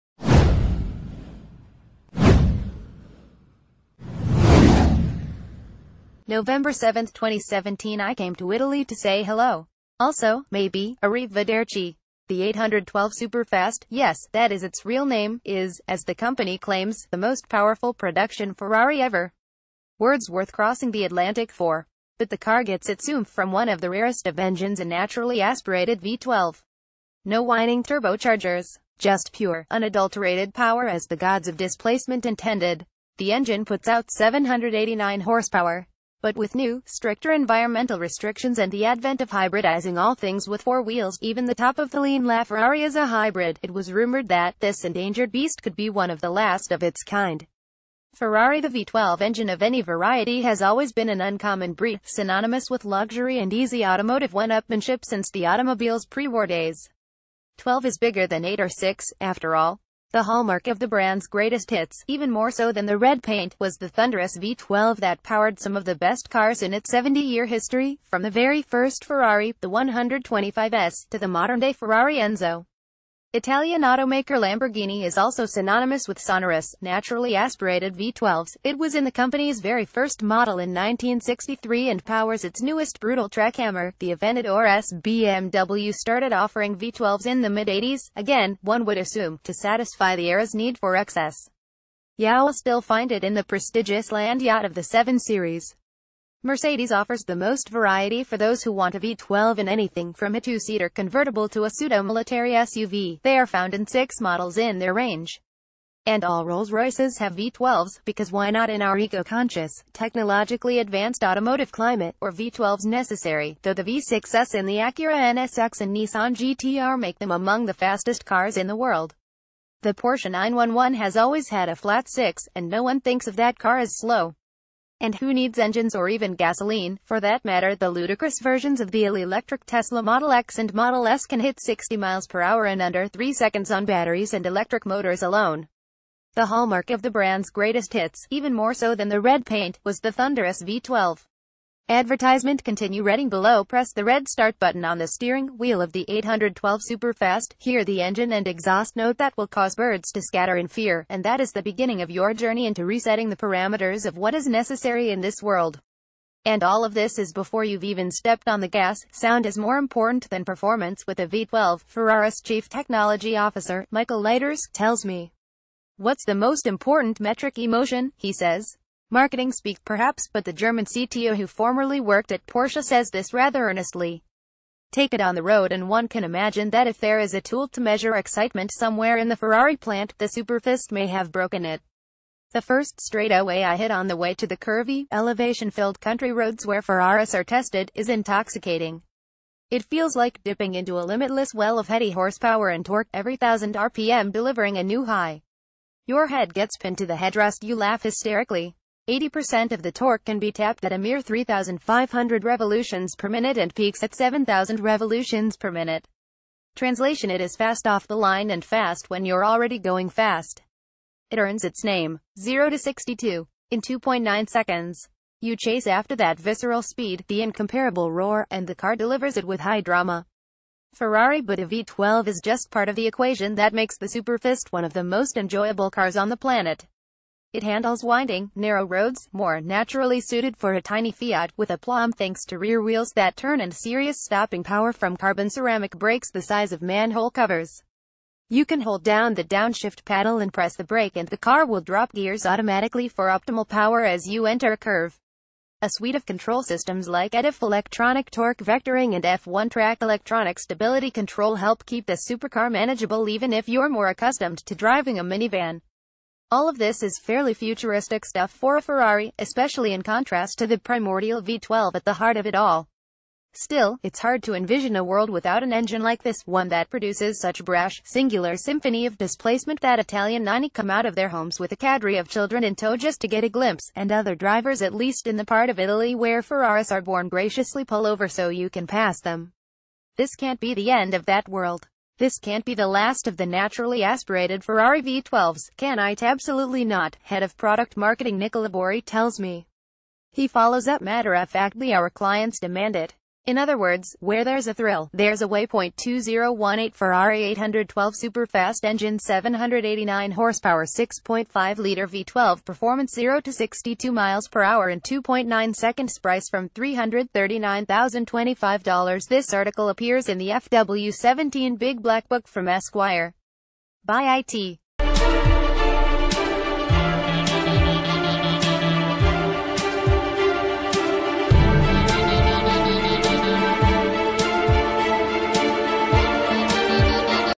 F1 | Is the 812 superfast the last ferrari with a v-12 engine?
Is the 812 superfast the last ferrari with a v-12 engine?

Nov 7, 2017 I came to Italy to say hello. Also, maybe, arrivederci. The 812 Superfast—yes, that is i...